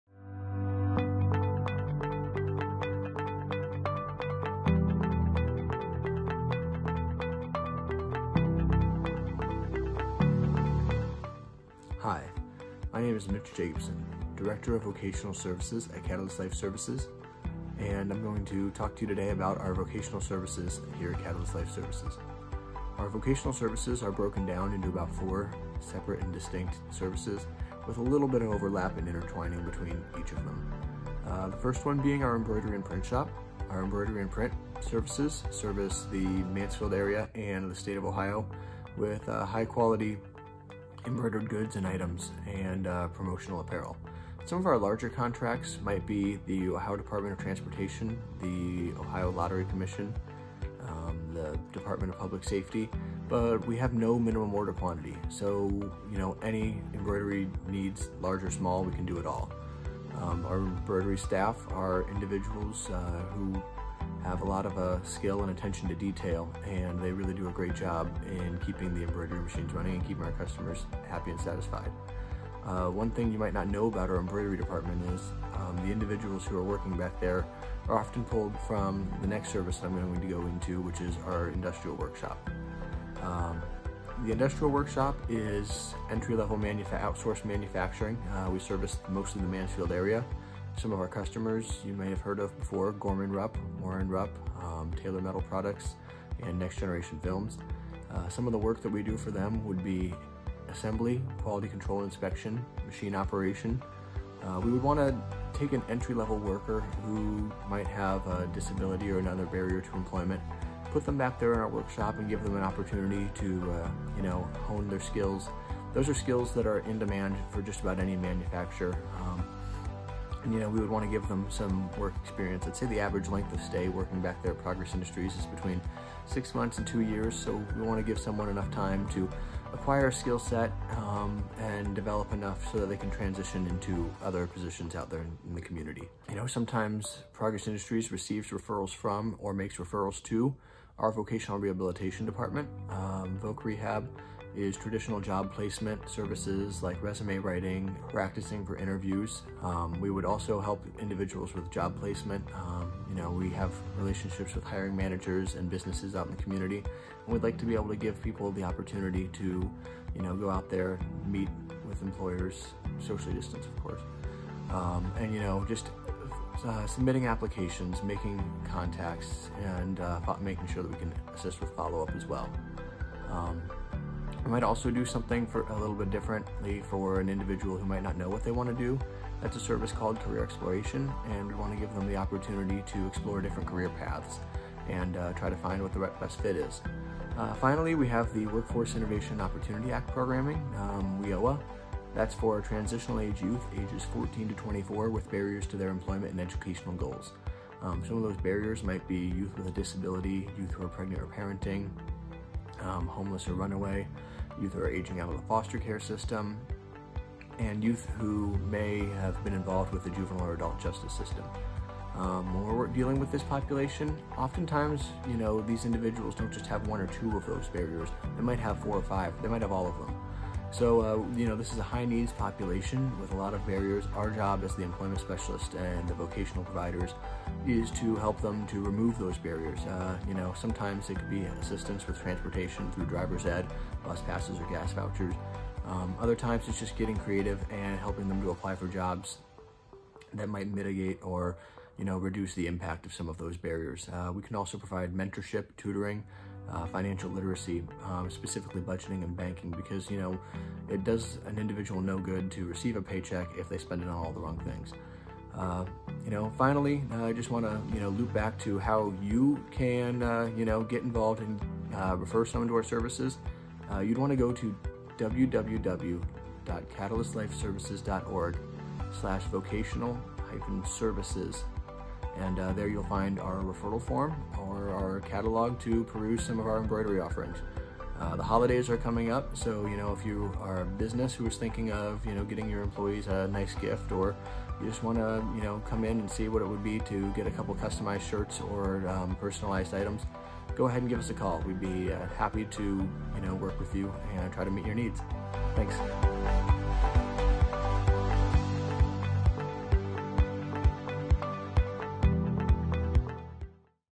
Agent of Change: Mitch Jacobsen
Hear from Mitch Jacobsen, Director of Vocational Services as he shares about the 4 main services of the Vocational Department. Over the past several decades, the vocational department has adapted and grown to serve the needs of our clients.